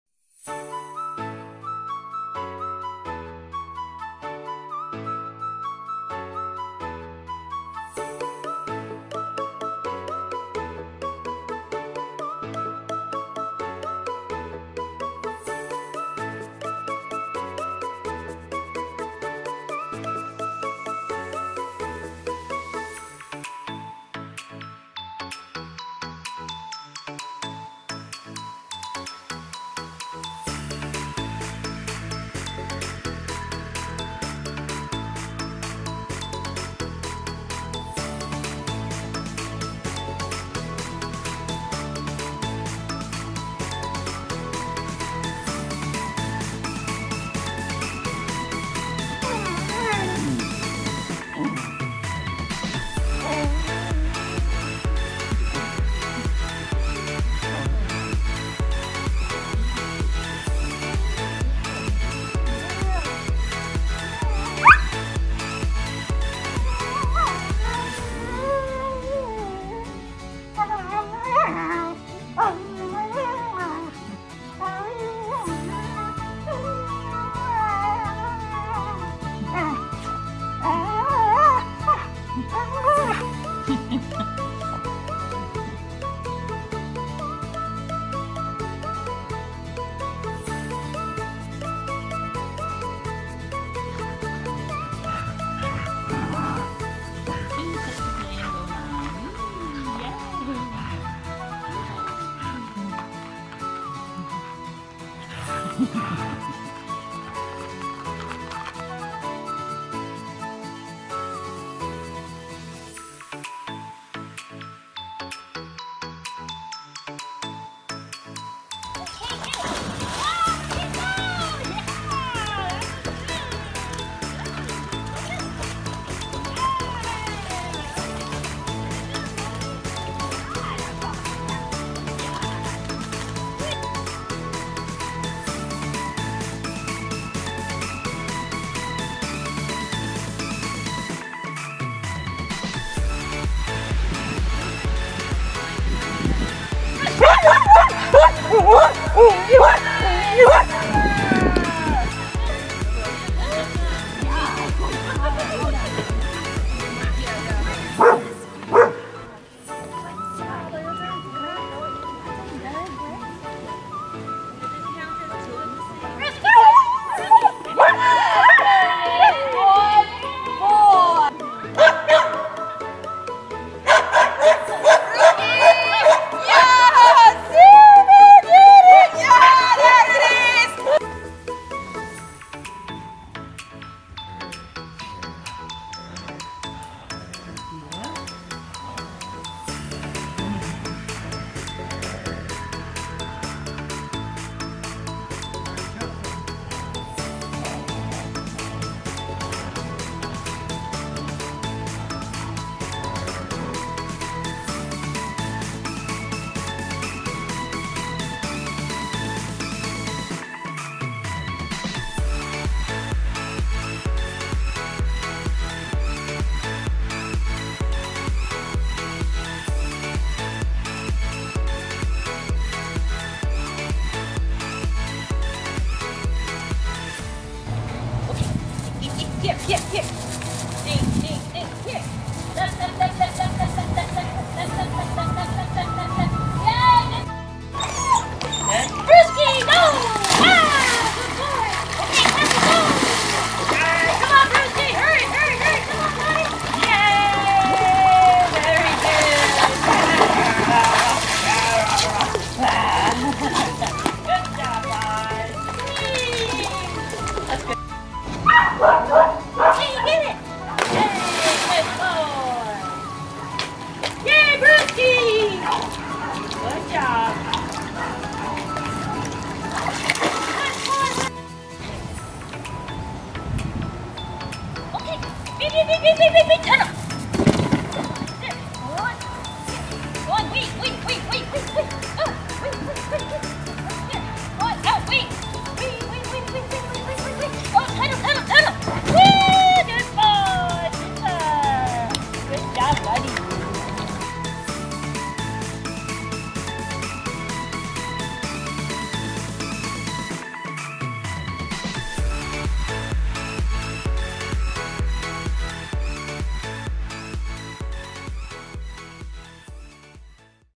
Brewsky turns one
A peek into the life of "Brewsky" in his first year